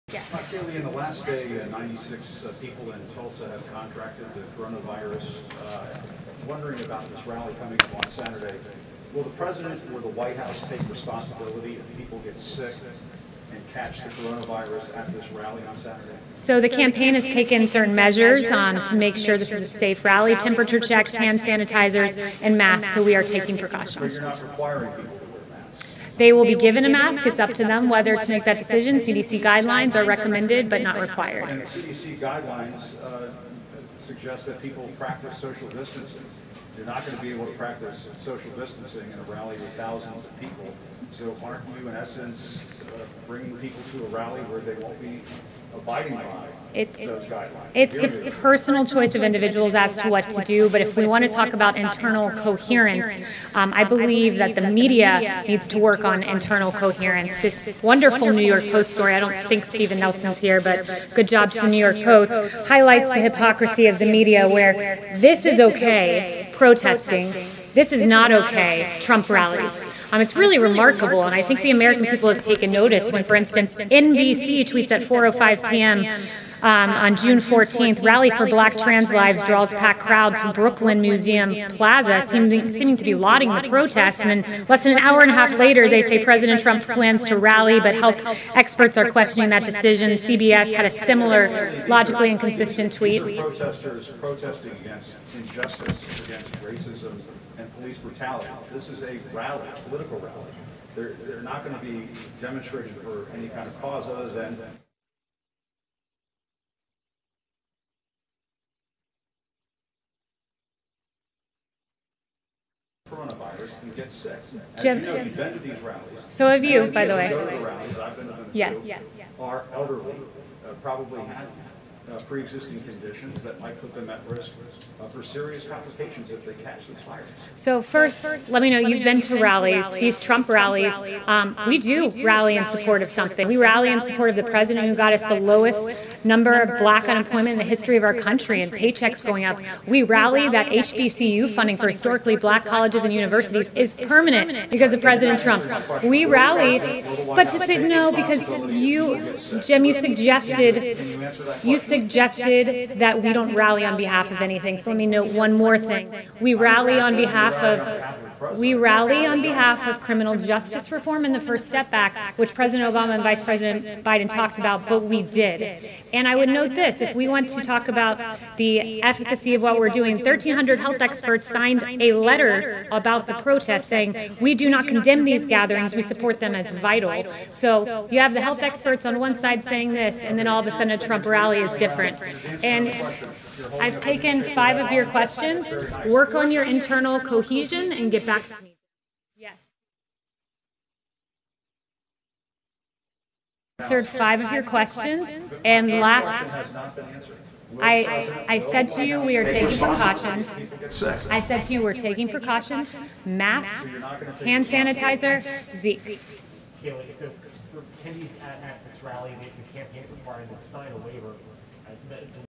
BLISTERING EXCHANGE: Press Secretary McEnany SHUTS DOWN CNN's Jim Acosta over Trump Rallies
White House Press Secreatry Kayleigh McEnany got into a Blistering Echange with the Media where shut Shuts down CNN's Jim Acosta for Trying to spread misinformation about Trump Rallies

*THE AUDIO WAS FROM THE WHITE HOUSE NOT OUR FAULT*